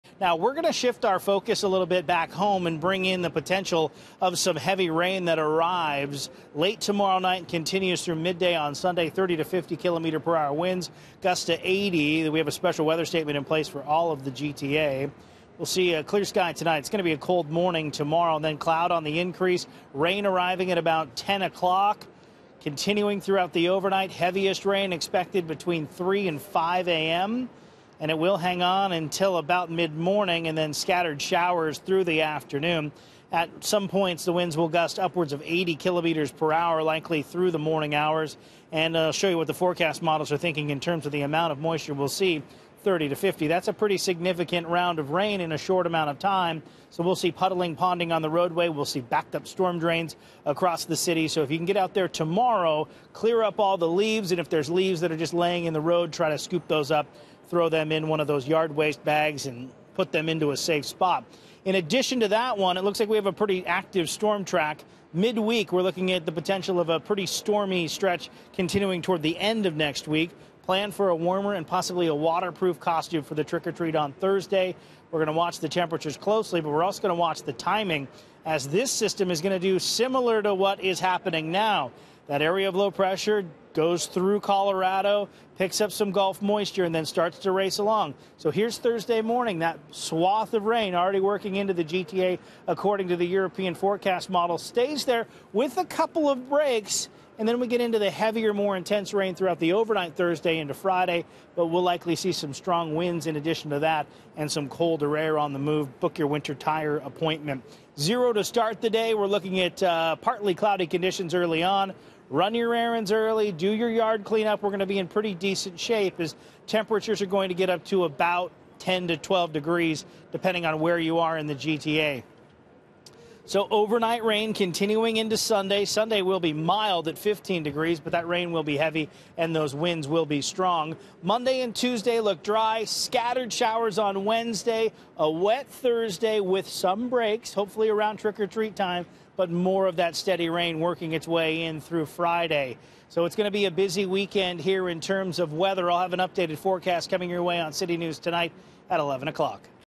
Heavy rain expected this weekend
Heavy rain is expected to begin Saturday night and continue through the weekend. Meteorologist Adam Stiles says we could see up to 50 mm of rain and gusting winds.